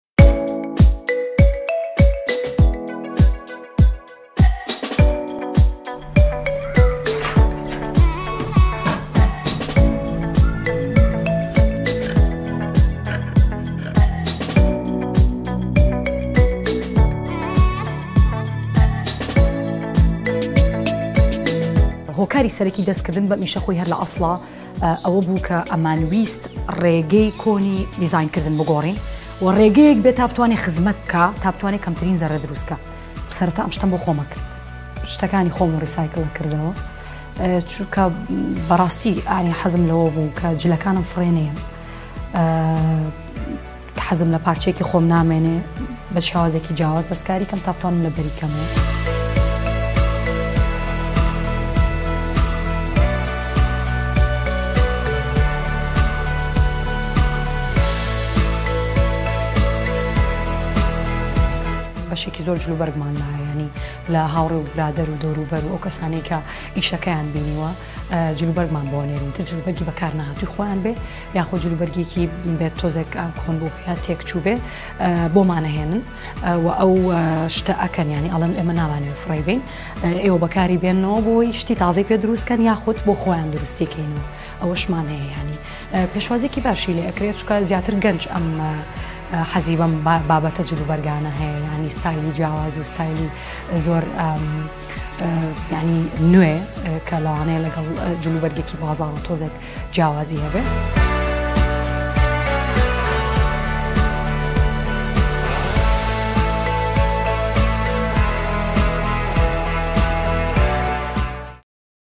Cette jeune Irakienne promeut la mode durable ! 😍👗
Cette styliste irakienne transforme les vêtements usés en belles créations! 😍👗 Regardez👇🏼